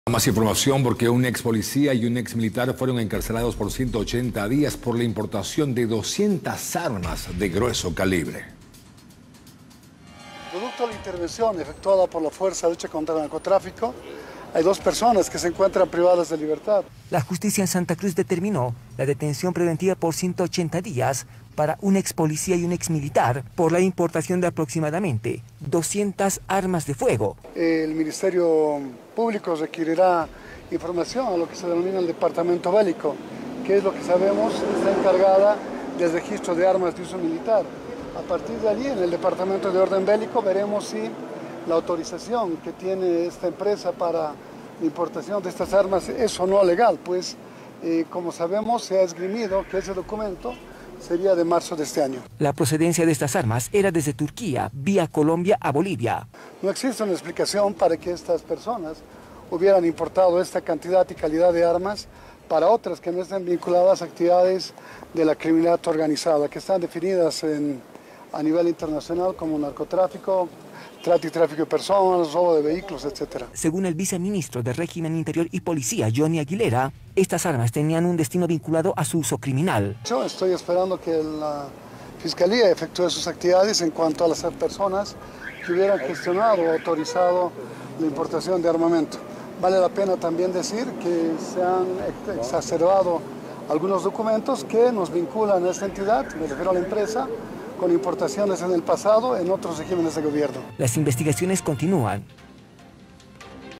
Expolicía y exmilitar son encarcelados por 180 días por la importación de armas de grueso calibre
▪️ Un expolicía y un exmilitar fueron encarcelados por 180 días por la importación de cerca de 200 armas de grueso calibre, las cuales tenían un destino vinculado a su uso criminal.